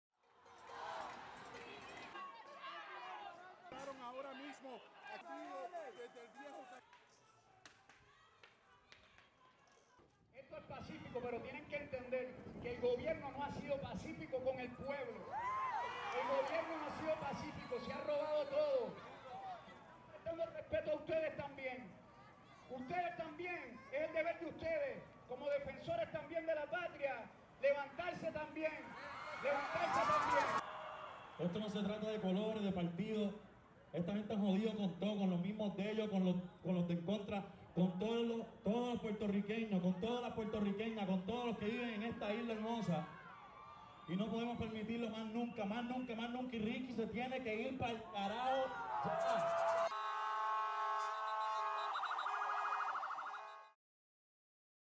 Police clash with protesters during demonstration in Puerto Rico | AFP
Police clash with protesters during a demonstration in the streets of San Juan, in Puerto Rico. Thousands gathered for a fifth day, demanding the resignation of Governor Ricardo Rossello, following corruption accusations and the leak of text chats in which he made sexist and homophobic remarks. "Ricky has to go now" said Puerto Rican reggaeton singer Bad Bunny, who with other local celebrity performers such as rapper Rene Perez, aka Residente, and singer Ricky Martin, took part in the demonstration.